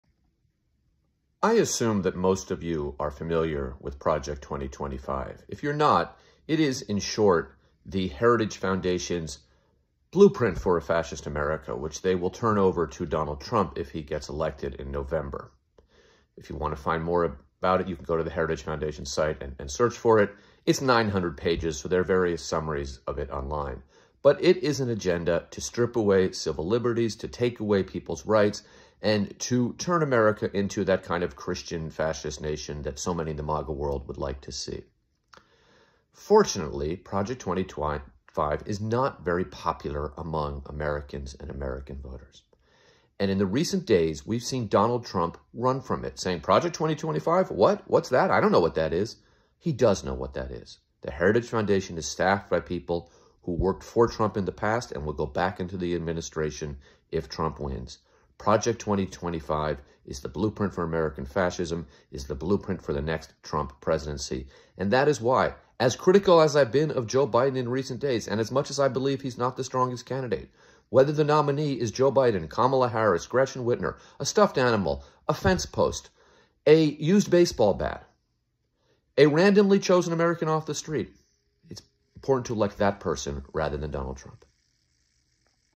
Mitchell Minute 1917-Donald Trump and Project 2025
July 12, 2024-Donald Trump has claimed not to know about Project 2025. That is yet another lie from Trump. Project 2025 was created by people with close ties to Trump. Those people will play key roles in his administration if Trump wins.